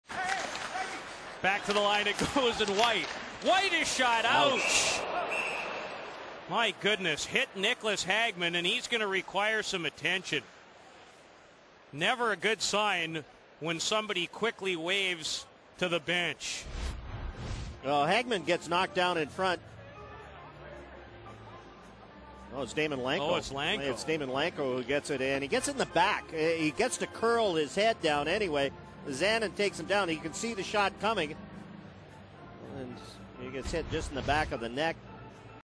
Daymond Langkow Take the Puck in the Neck 3/21/2010
Daymond Langkow gets shoved and as he is falling, he ducks his head when he sees a shot coming from the point and the shot nails him in the back of the neck. He had to be taken off on a stretcher after lying on the ice for several minutes. (Calgary Flames @ Minnesota Wild - March 21, 2010)